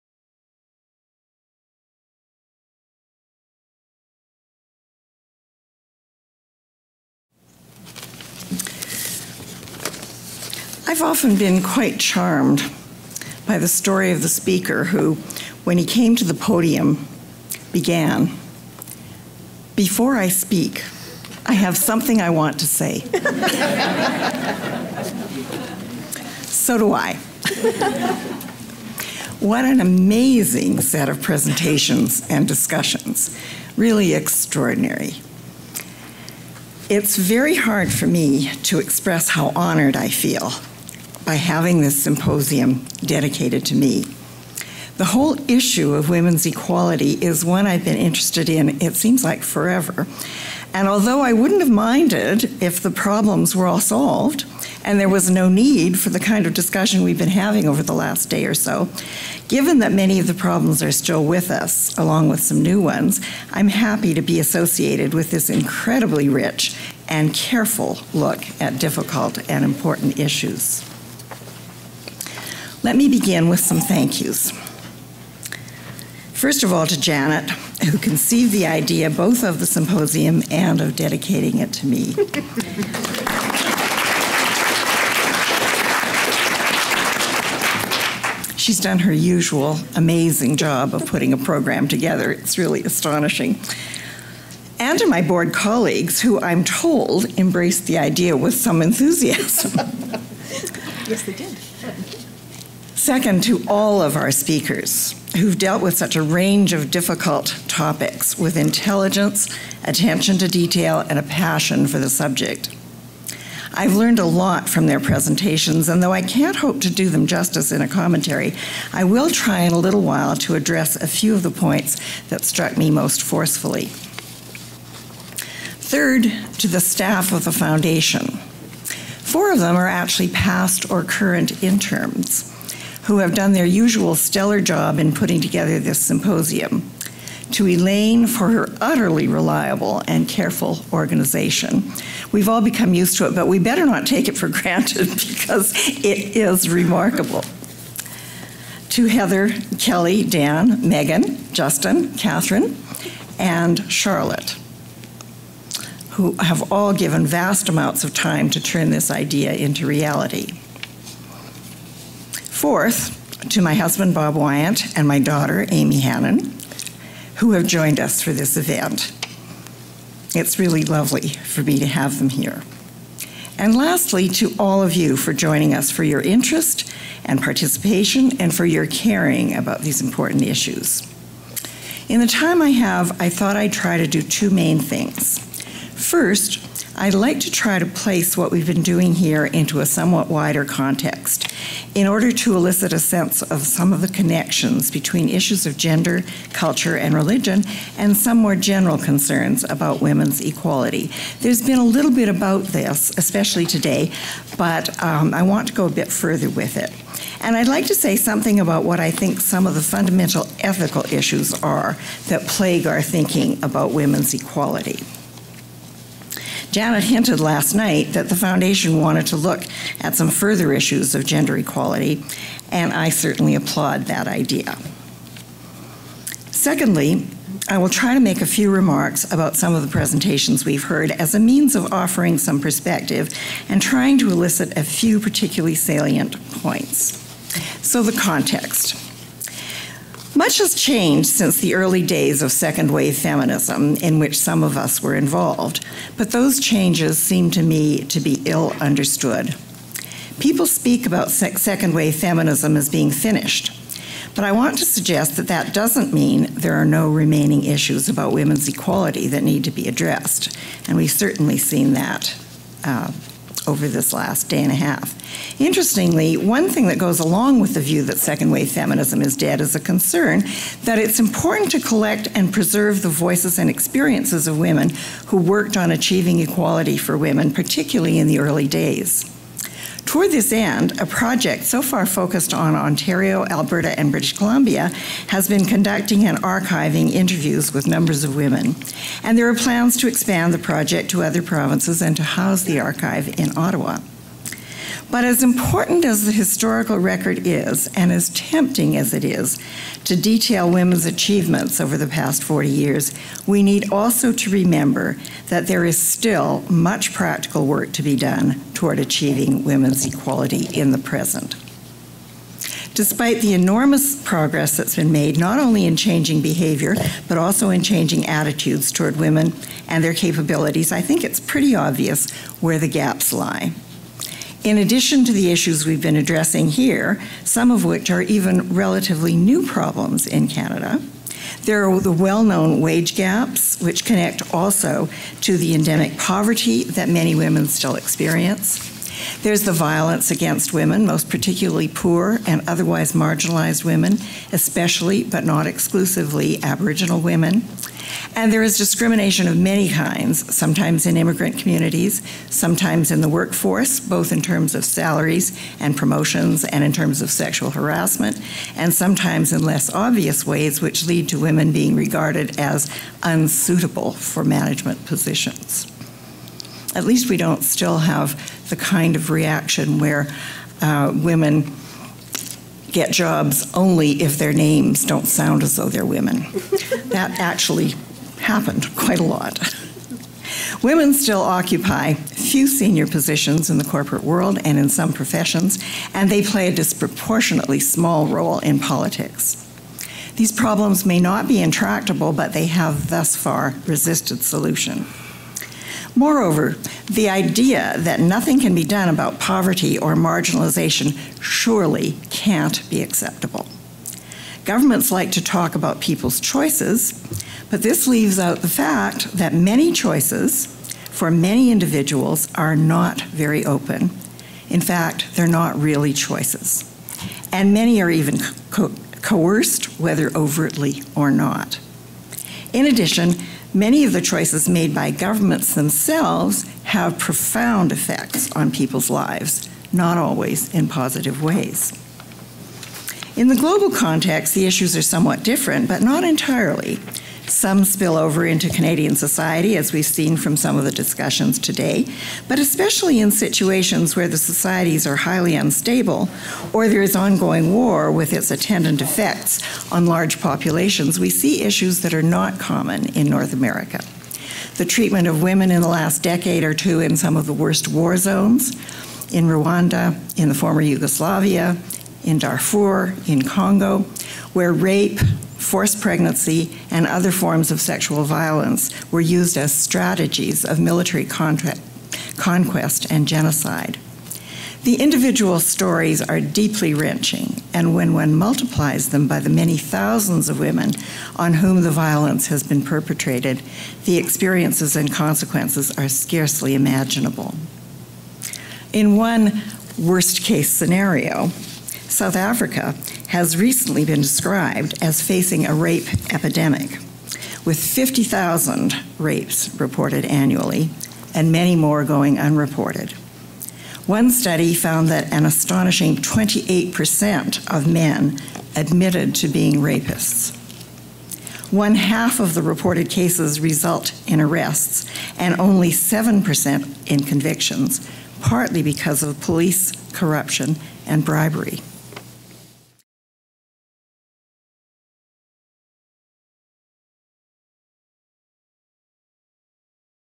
Gender, Culture and Religion - Reflections by Marsha Hanen - 1 of 3
Marsha Hanen gives reflections on the Sheldon Chumir Foundation Symposium: Gender, Culture and Religion: Tackling some difficult questions